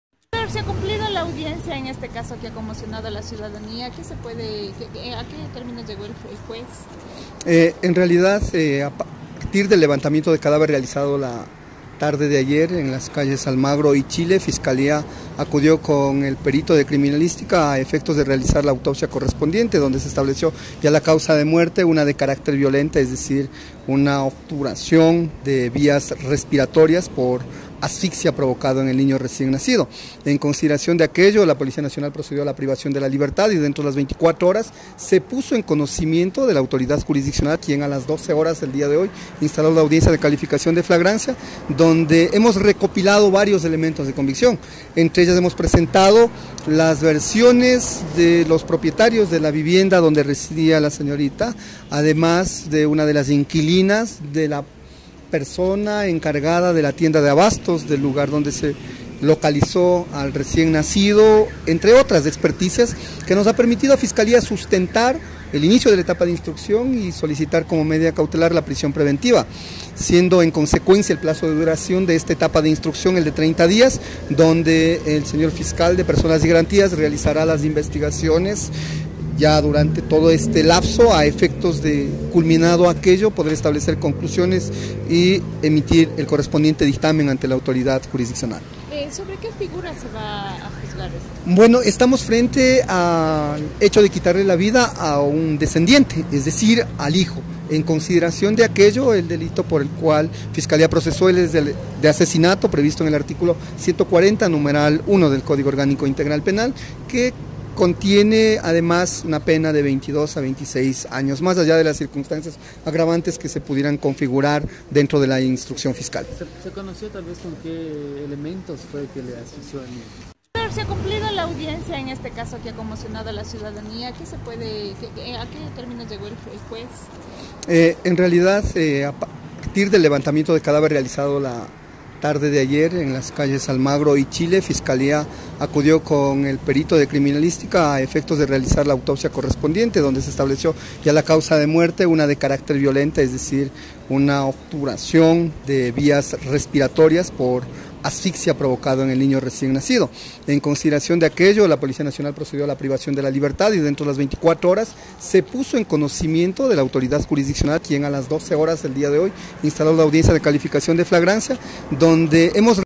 La madre del bebé que fue hallado sin vida en una maleta se encuentra con prisión preventiva.
La madre del bebé que fue hallado sin vida en una maleta se encuentra con prisión preventiva. Fiscalía y Policía Nacional continúan con las investigaciones.